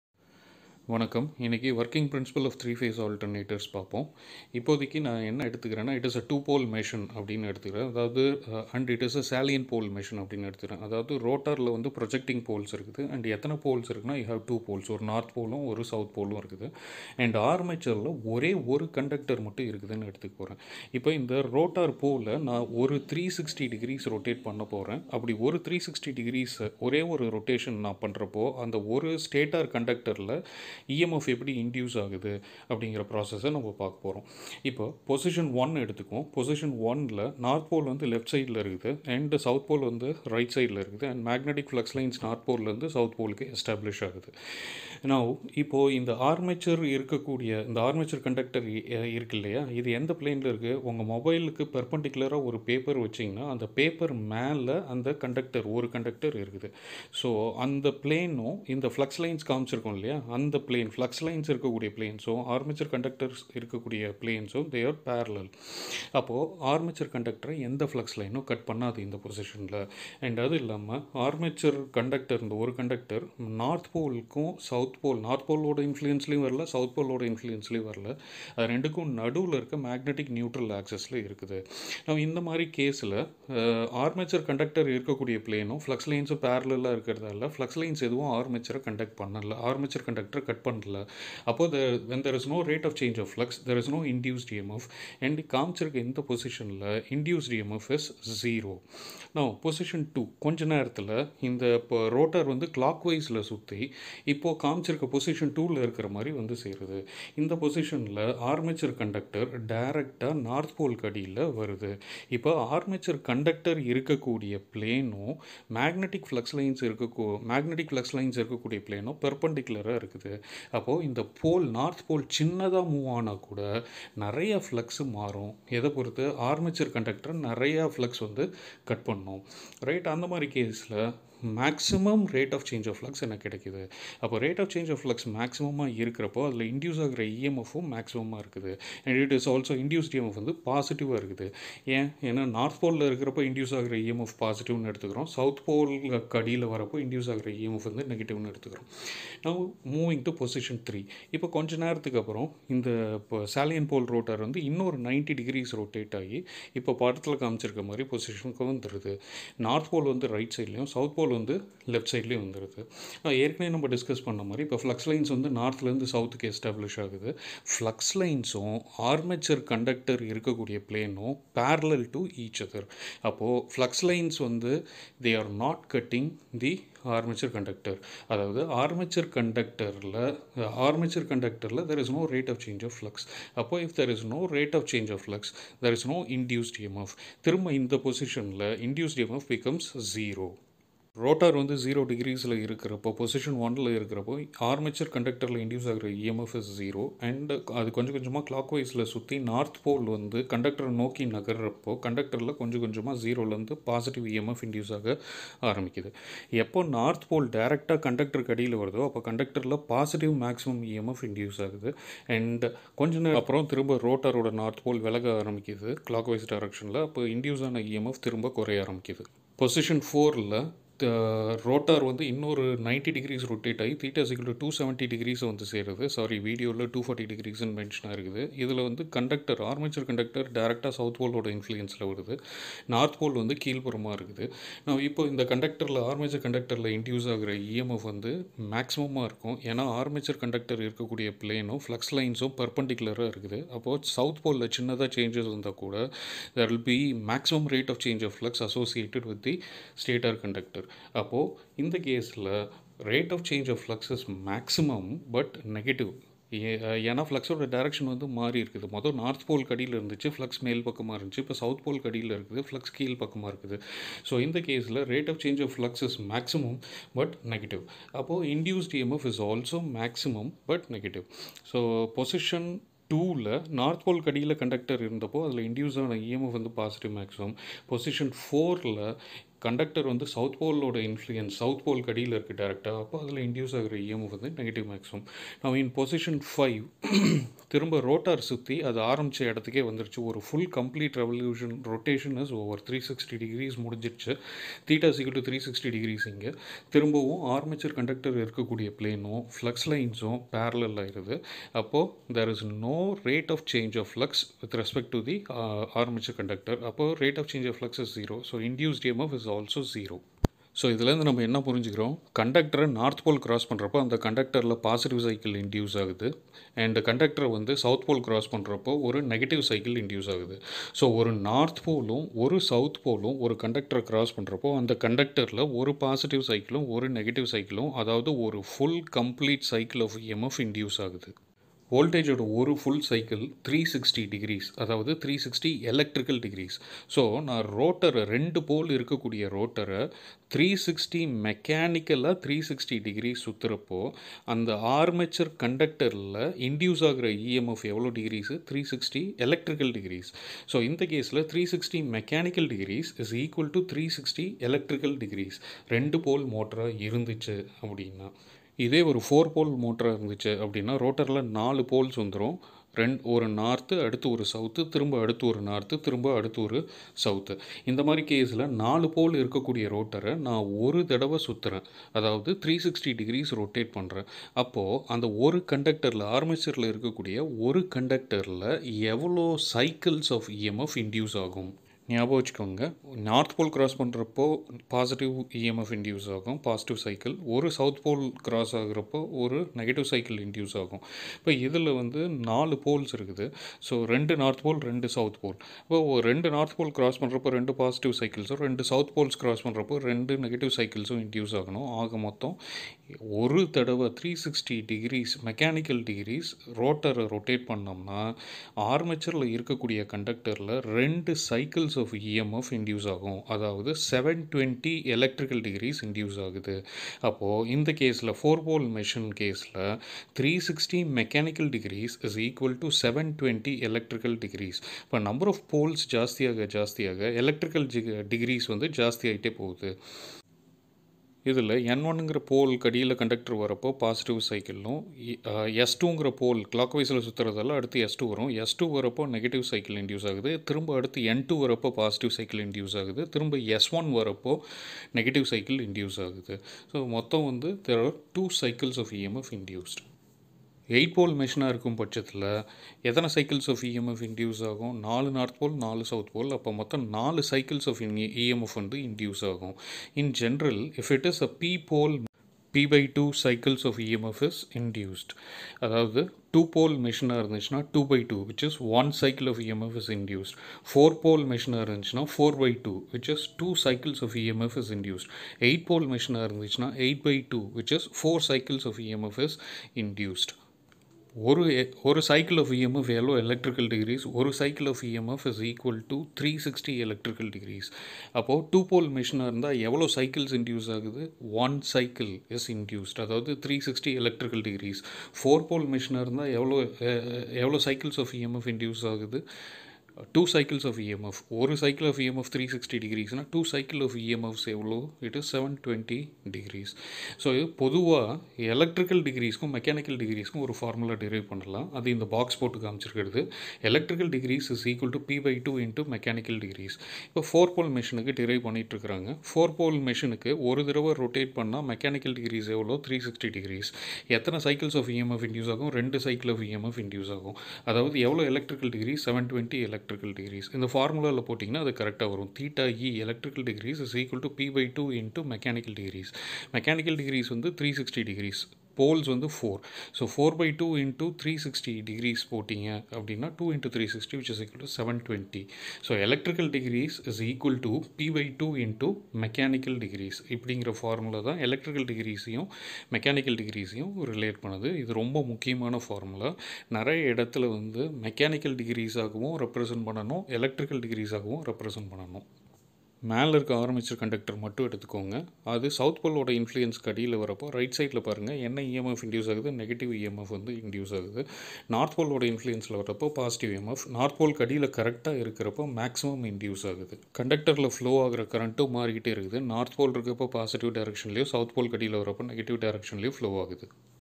Alternators - working principle || alternator lecture 5
The working principle of a 2 pole salient pole alternator considering only one armature conductor in armature is explained. Also the relationship between electrical and mechanical degrees is derived and explained.